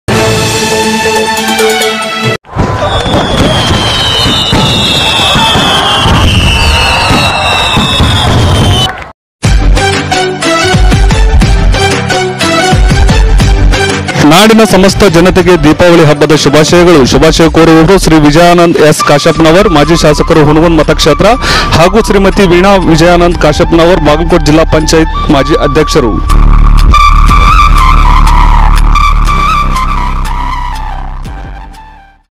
ನಾಡಿನ ಸಮಸ್ತ ಜನತೆಗೆ ದೀಪಾವಳಿ ಹಬ್ಬದ ಶುಭಾಶಯಗಳು
ಶುಭ ಕೋರುವರು ಶ್ರೀ ವಿಜಯಾನಂದ ಎಸ್ ಕಾಶಪ್ಪನವರ್ ಹುನುಗುಂದ ಮತಕ್ಷೇತ್ರದ ಮಾಜಿ ಶಾಸಕರು ಹಾಗೂ ಶ್ರೀ ವೀಣಾ ವಿಜಯಾನಂದ ಕಾಶಪ್ಪನವರ್ ಬಾಗಲಕೋಟ್ ಜಿಲ್ಲಾ ಪಂಚಾಯತ್ ಮಾಜಿ ಅಧ್ಯಕ್ಷರು